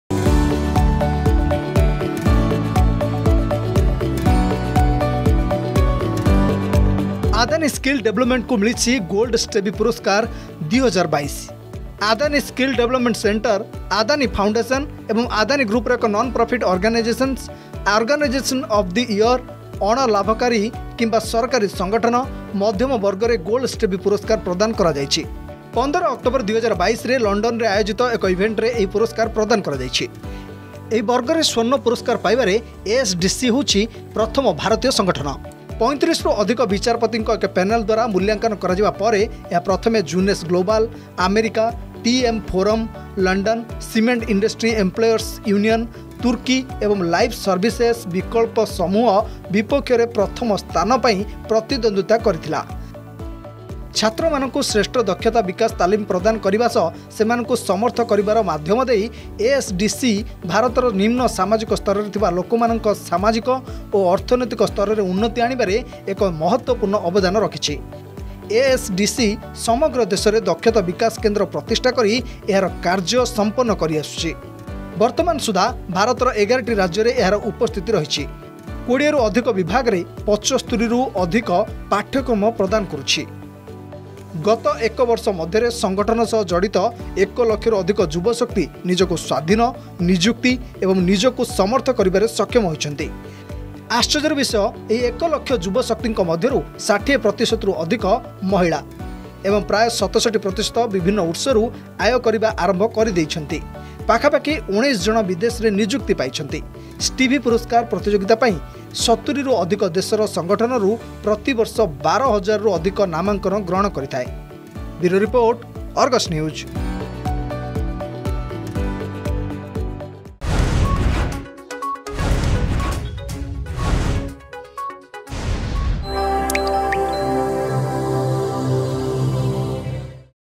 ଆଦାନୀ ସ୍କିଲ୍ ଡେଭଲପମେଣ୍ଟକୁ ମିଳିଛି ଗୋଲ୍ଡ ଷ୍ଟେଭି ପୁରସ୍କାର 2022
ଆଦାନୀ ସ୍କିଲ୍ ଡେଭଲପମେଣ୍ଟକୁ ମିଳିଛି ଗୋଲ୍ଡ ଷ୍ଟେଭି ପୁରସ୍କାର 2022।  ଆଦାନୀ ସ୍କିଲ୍ ଡେଭଲପମେଣ୍ଟ ସେଣ୍ଟର (ASDC), ଆଦାନୀ ଫାଉଣ୍ଡେସନ ଏବଂ ଆଦାନୀ ଗ୍ରୁପର ଏକ ନନ ପ୍ରଫିଟ୍ ଅର୍ଗାନେଇଜେନସ, 'ଅର୍ଗାନାଇଜେସନ ଅଫ୍ ଦି ଇୟର - ଅଣ-ଲାଭକାରୀ କିମ୍ବା ସରକାରୀ ସଂଗଠନ - ମଧ୍ୟମ ବର୍ଗରେ ଗୋଲ୍ଡ ଷ୍ଟେଭି ପୁରସ୍କାର ପ୍ରଦାନ କରାଯାଇଛି।